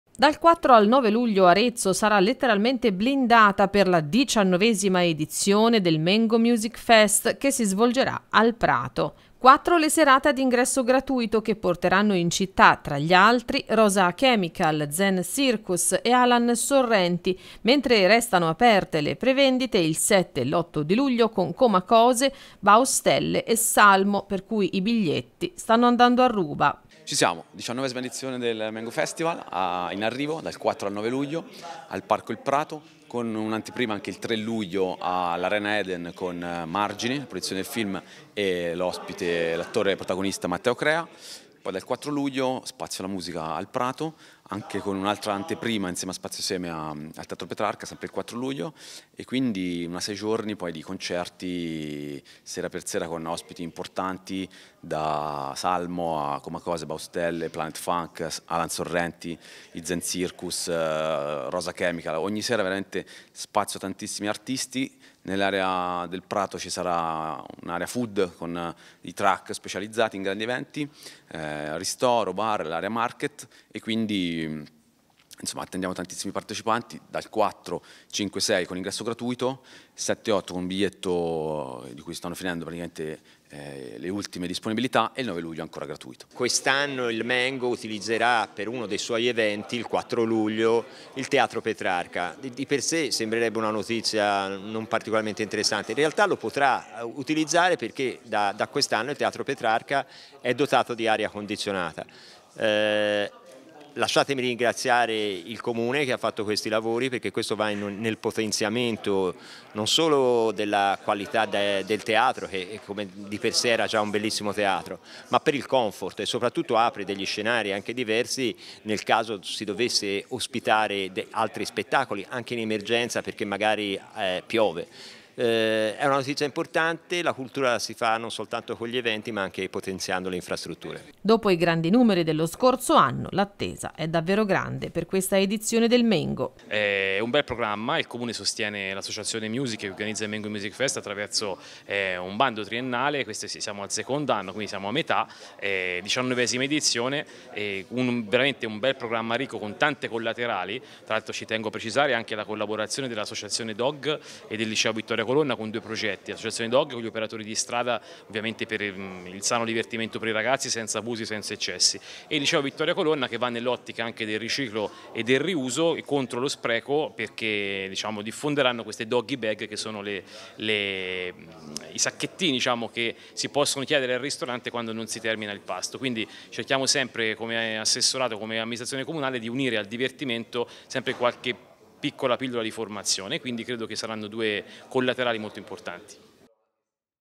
Torna il Mengo Music Fest ad Arezzo: tutto pronto per le 19esima edizione
Manca sempre meno a MEN/GO MUSIC FEST: dal 4 al 9 luglio torna la grande musica a PARCO IL PRATO di AREZZO.  Sei giorni con i migliori artisti della scena, un parterre di tutto rispetto con i tour più attesi dell’estate. Dal pop al rock, dal rap al folk fino all’elettronica, al MEN/GO c’è spazio per tutti i generi musicali con una sola parola d’ordine: trasversalità.